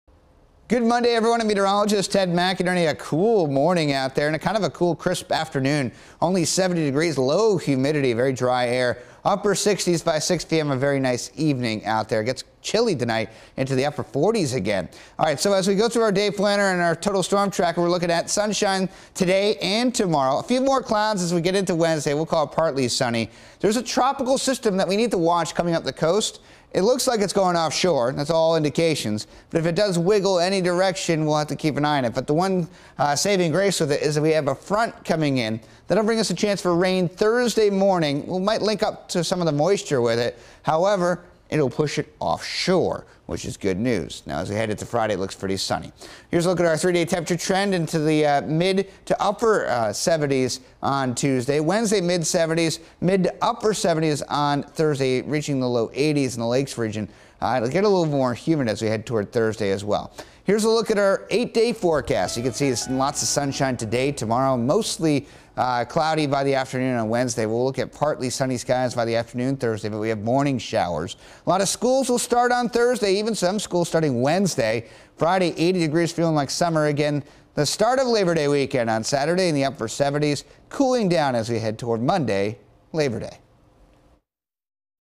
Staying dry most of the week
How’s the weather looking for your Monday afternoon? Get the latest Maine’s Total Weather video forecast from Meteorologist Ted McInerney.